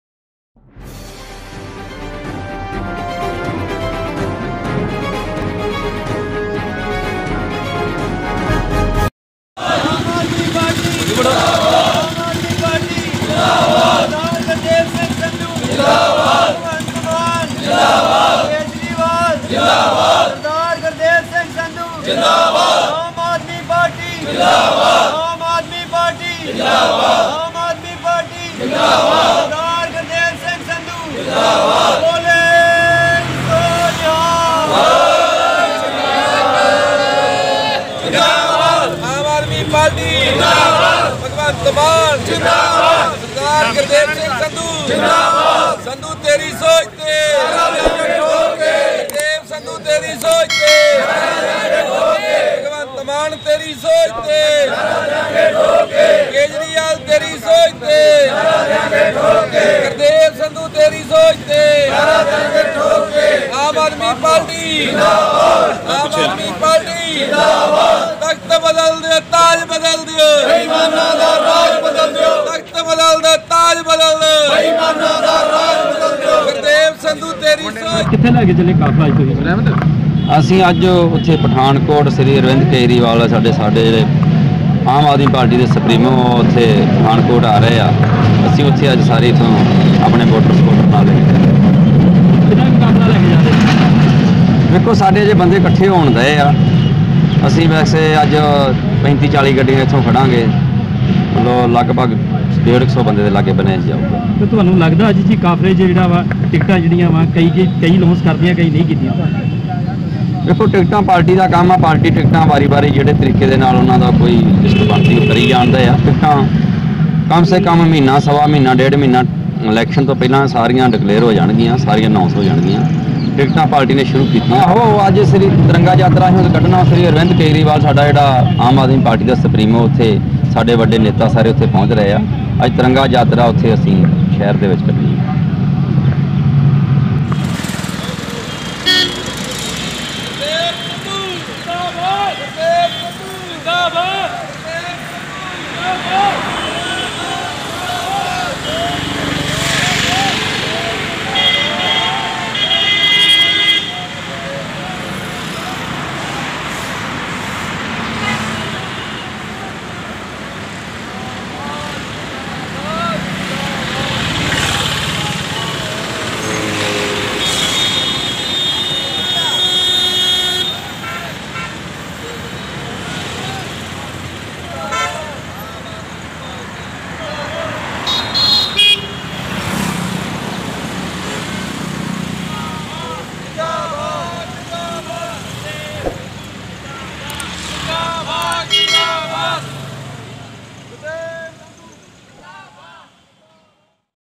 ਆਮ ਆਦਮੀ ਪਾਰਟੀ ਦੇ ਗੁਰਦੇਵ ਸਿੰਘ ਸੰਧੂ ਵੱਲੋਂ ਬਹੁਤ ਵੱਡਾ ਜਥਾ ਲੈ ਕੇ ਪਠਾਨਕੋਟ ਨੂੰ ਹੋਏ ਰਵਾਨਾ
ਆਮ ਆਦਮੀ ਪਾਰਟੀ ਗੁਰਦੇਵ ਸਿੰਘ ਸੰਧੂ ਵੱਲੋਂ ਬਹੁਤ ਵੱਡਾ ਜਥਾ ਲੈ ਕੇ ਰਵਾਨਾ ਹੋਏ ਪਠਾਨਕੋਟ ਲਈ ਜਥੇ ਆਮ ਆਦਮੀ ਪਾਰਟੀ ਕੇਜਰੀਵਾਲ ਵੱਲੋਂ ਤਿਰੰਗਾ ਯਾਤਰਾ ਕੱਢੀ ਜਾ ਰਹੀ ਹੈ